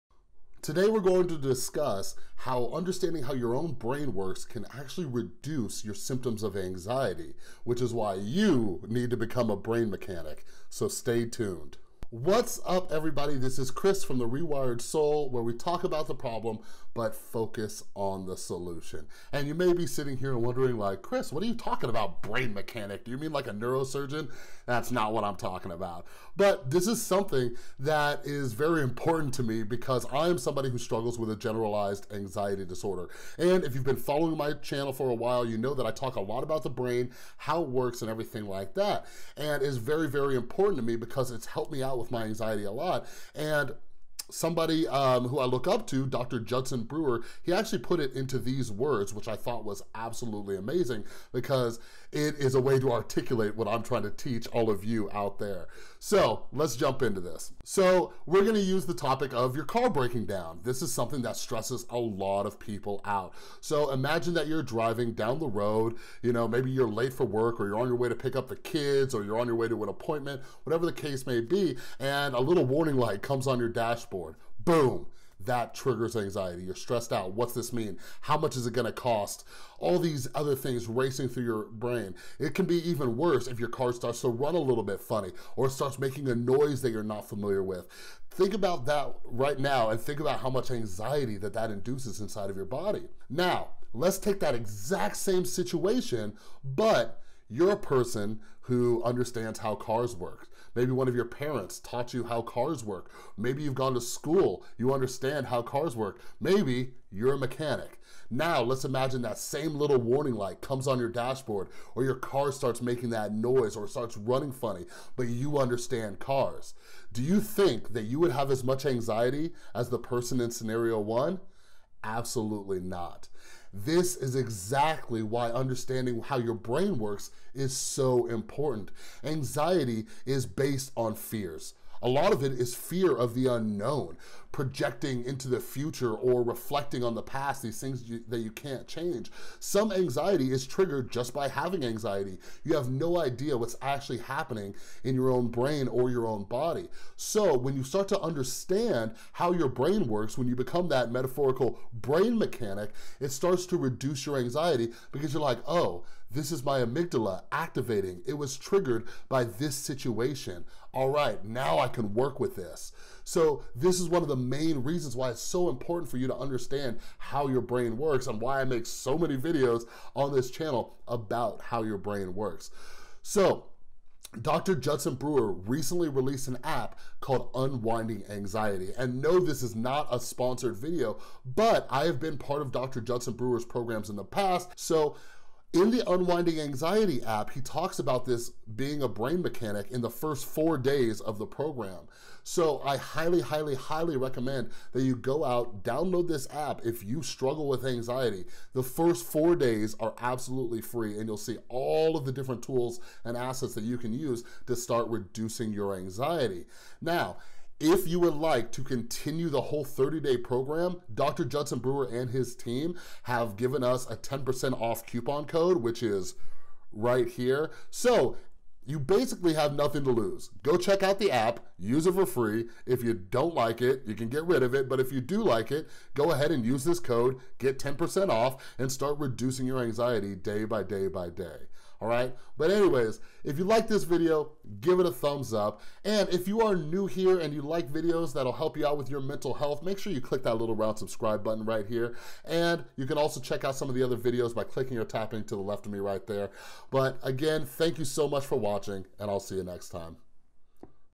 How to Reduce Anxiety and Stress by Becoming a "Brain Mechanic"
You can learn how to reduce anxiety and stress by becoming a "brain mechanic". This video discusses how understanding your own brain is going to be the most effective way to begin overcoming your symptoms of anxiety.